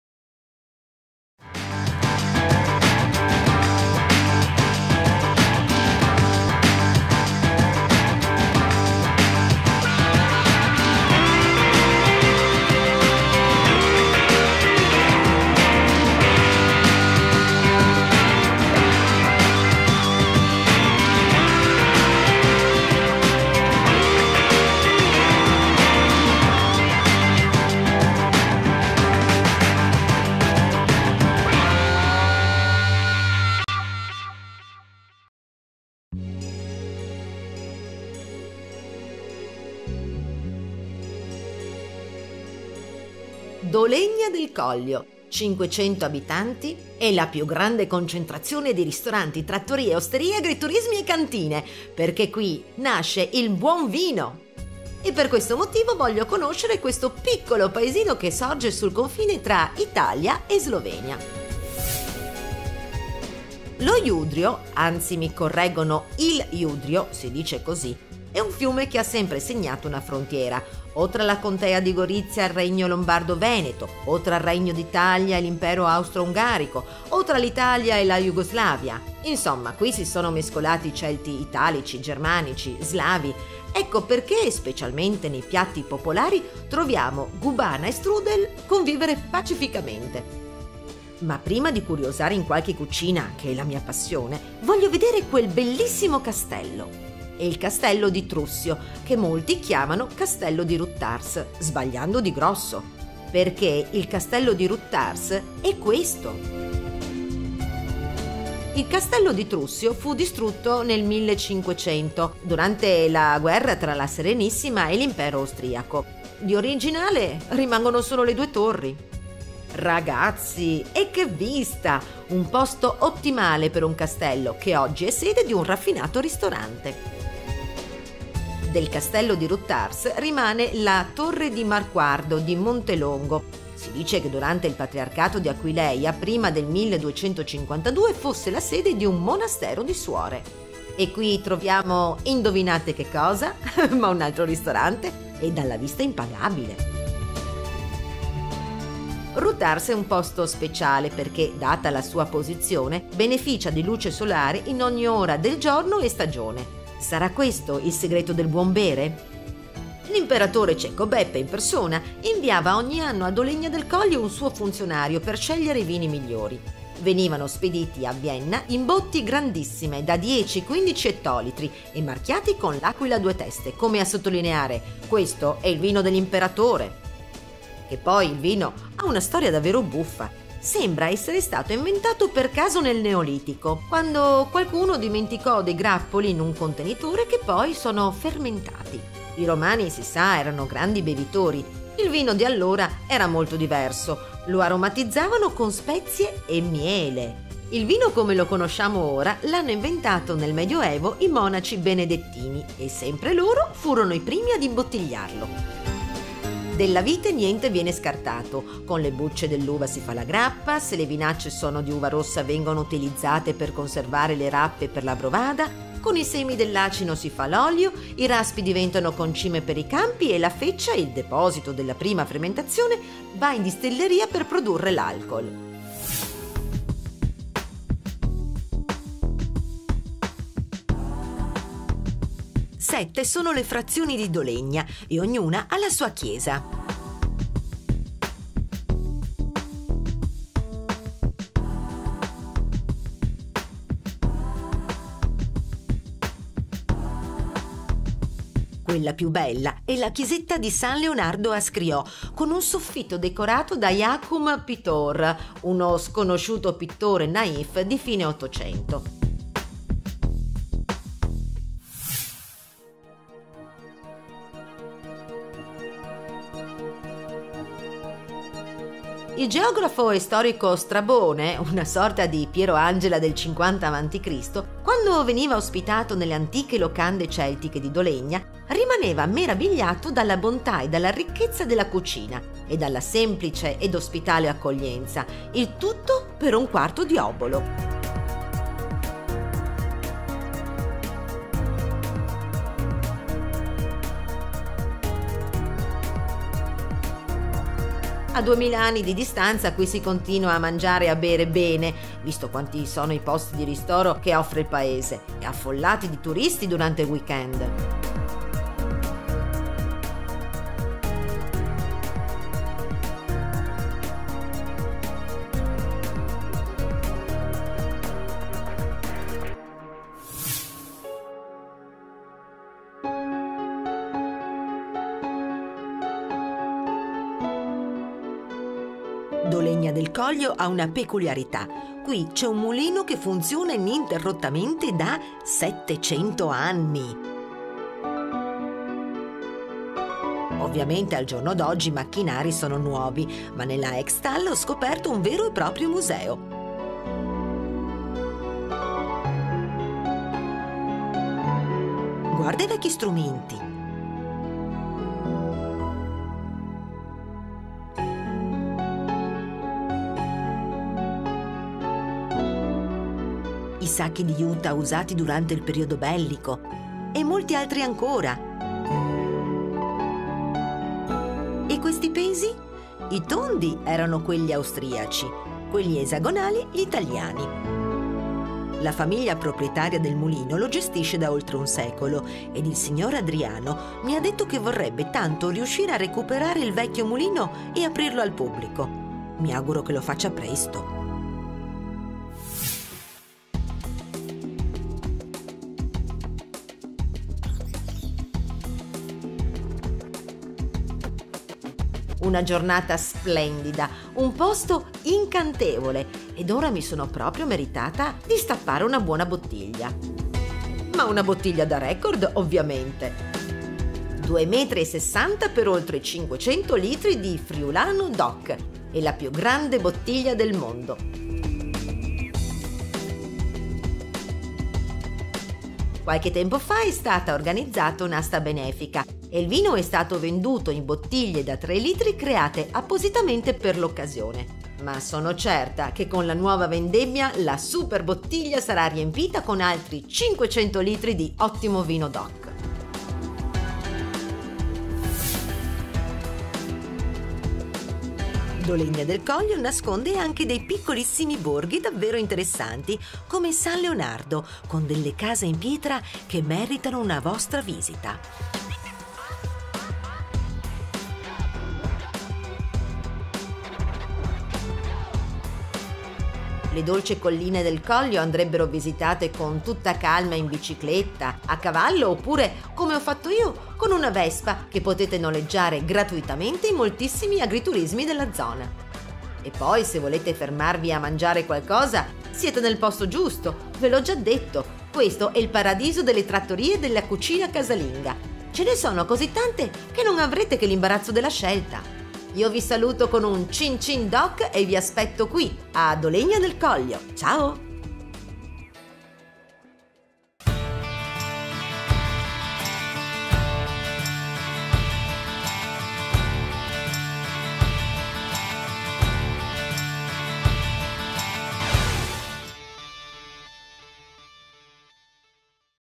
dolegna del collio
la bella dolega del collio patria di agriturismi e ottimo vino, ma non solo.